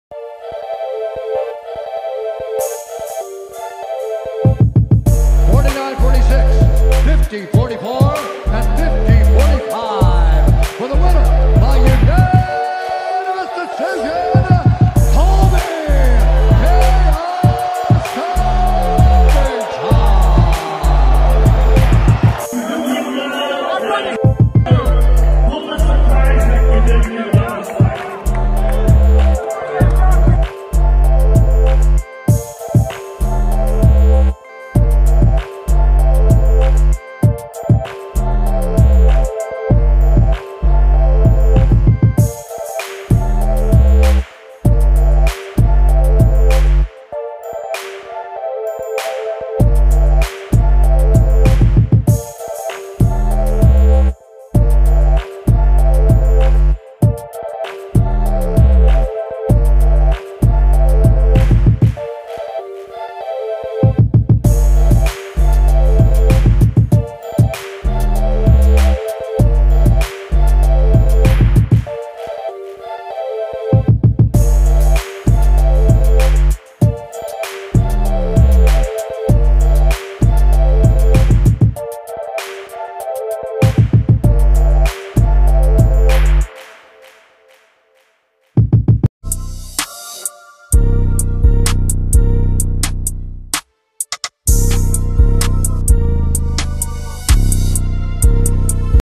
مقاتلوا ufc يعلقون على انتصار كولبي كوفينغتون بعد سيطرته الكاملة على القتال ضد خورخي ماسفيدال،ufc272
مقاتلوا ufc يعلقون على انتصار كولبي كوفينغتون بعد سيطرته الكاملة على القتال ضد خورخي ماسفيدال